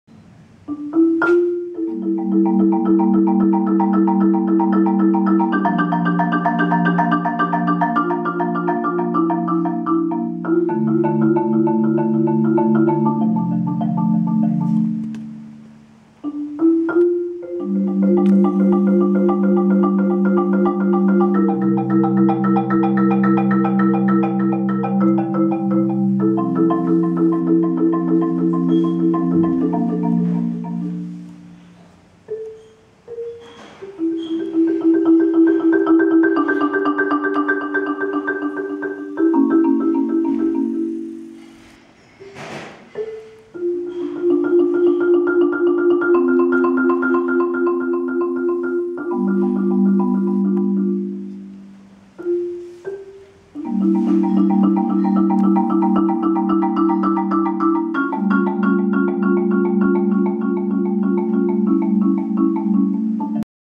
Marimbaphone
Hochzeitskonzert für den Patenonkel am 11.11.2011 in Mainz